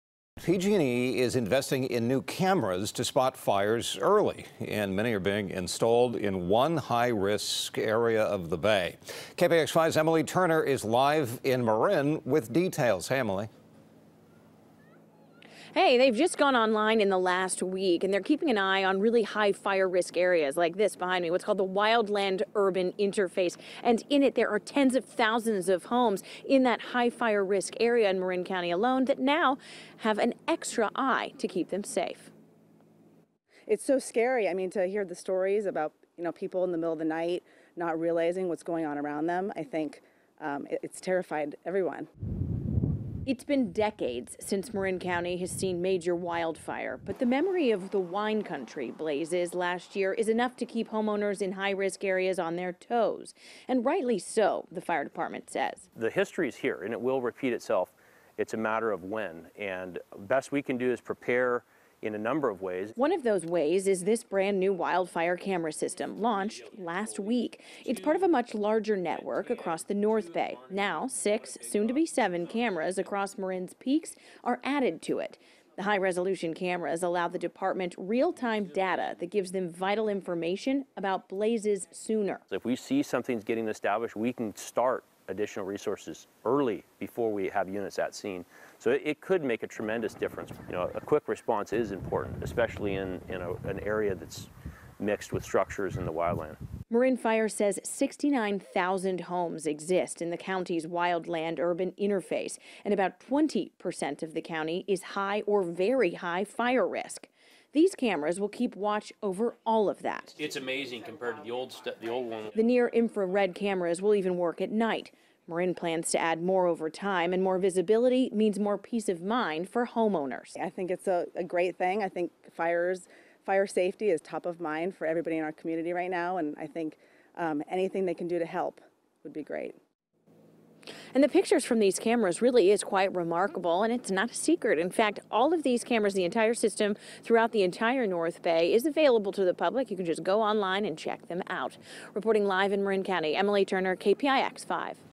PG&E Installs High-Def Camera Network to Monitor Marin Hills for Fire
The utility blamed for almost a dozen California wildfires is giving a big gift to Marin County -- company has paid for a network of new fire-monitoring cameras to go up on the county’s peaks. Emily Turner reports. (12-25-18)